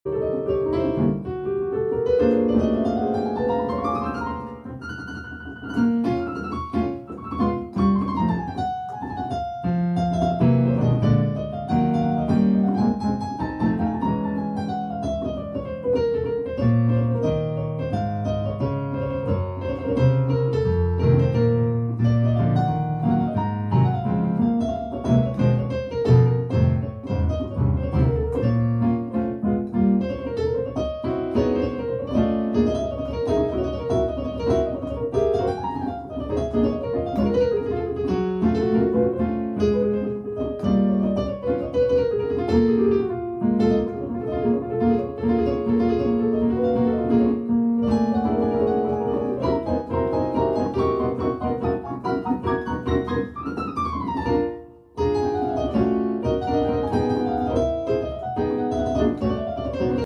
Free Jazz Piano Improvising jazz piano(5)
Free Jazz Piano Solo Improvisation on Roland Keyboard by Huddie Sepehrmanesh